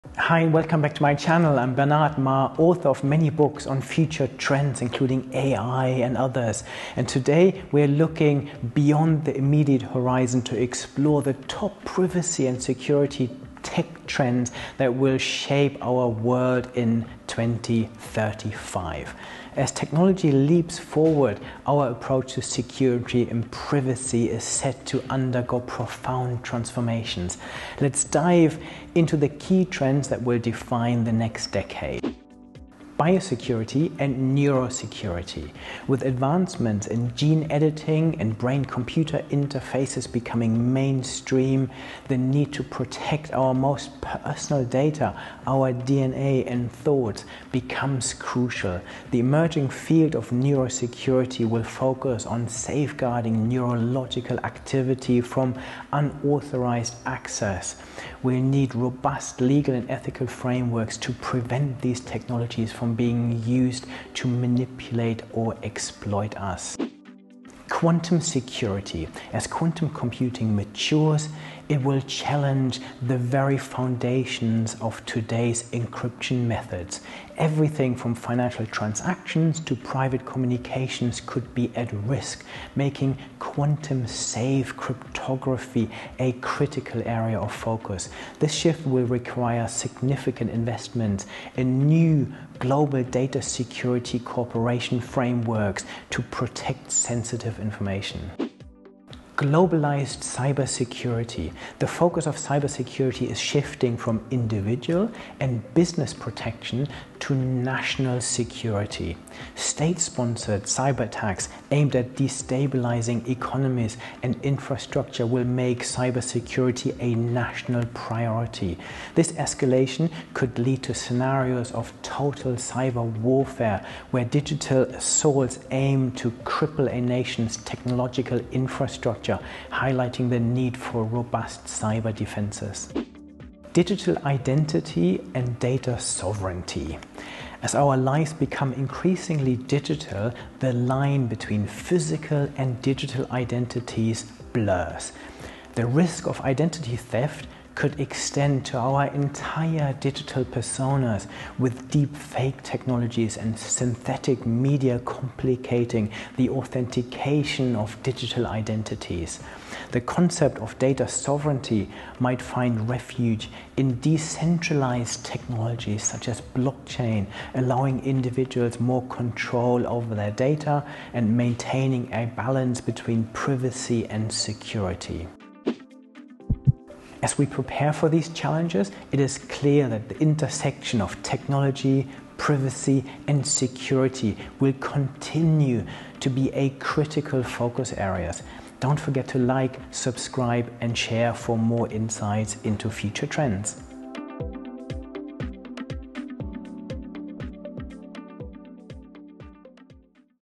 The Privacy & Security Trends For The Next Decade
Explore the future of privacy and security! 🔒 Learn about key trends like biosecurity, quantum security, and globalized cybersecurity that will shape our world by 2035. Understand the importance of protecting our DNA, thoughts, and digital identities as technology advances. Stay ahead of the curve with insights on the intersection of technology, privacy, and security.